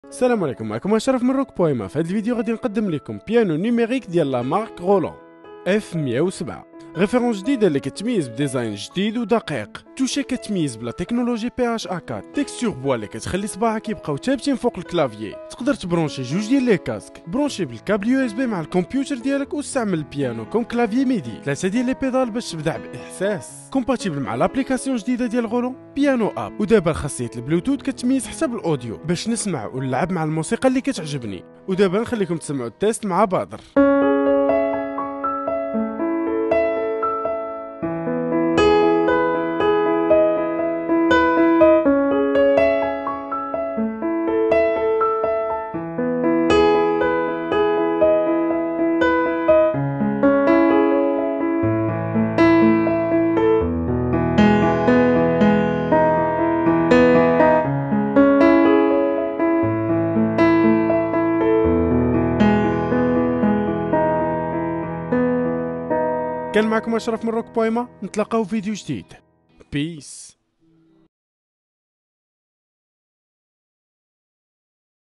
Roland F107 (arabe) - Rock.ma
Présentation du piano @rolandglobal F107 chez RockMa Musique au Maroc!

RockMa Musique! le plus grand magasin d'instruments de musique au Maroc